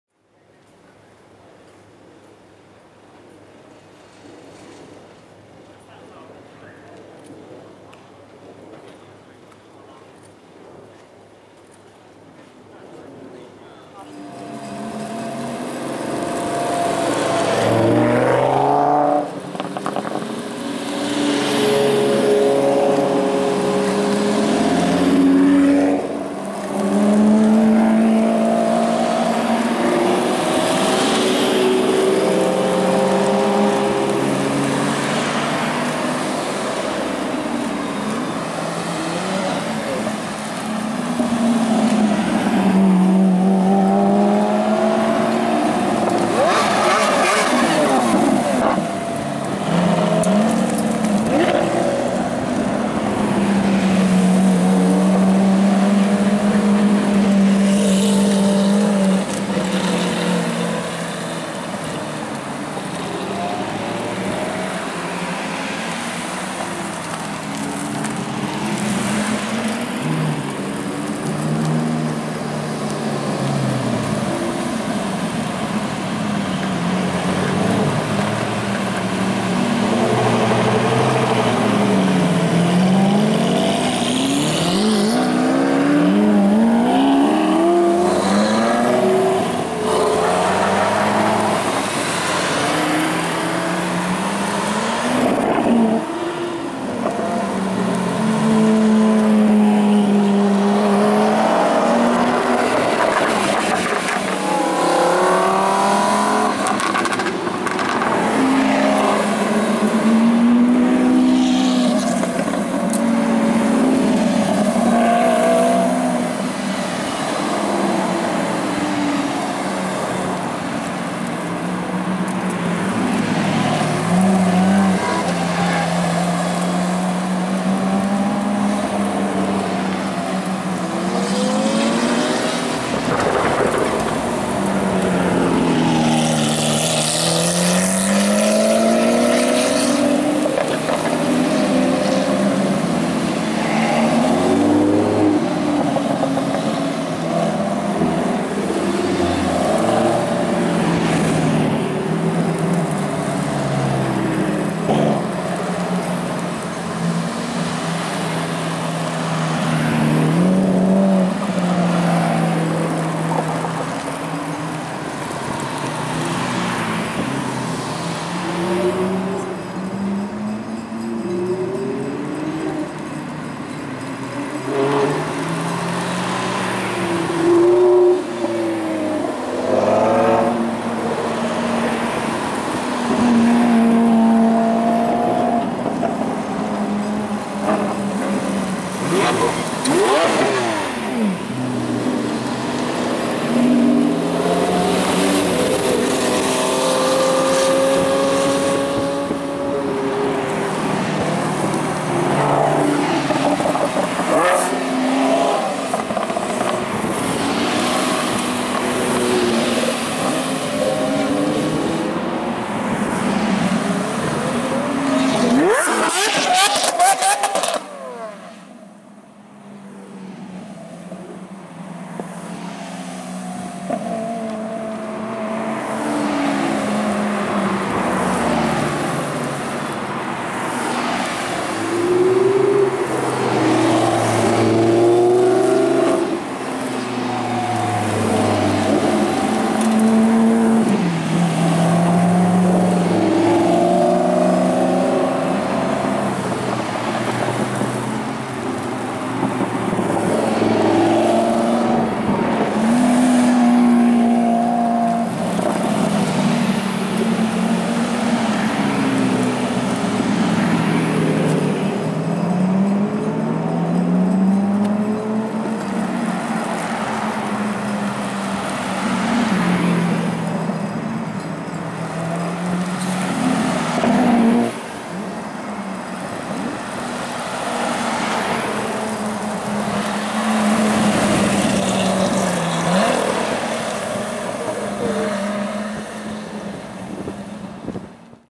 100 Lamborghinis Convoy Departure
Maxson Goh Films 100 Lamborghinis departing Changi Exhibition Centre in a convoy.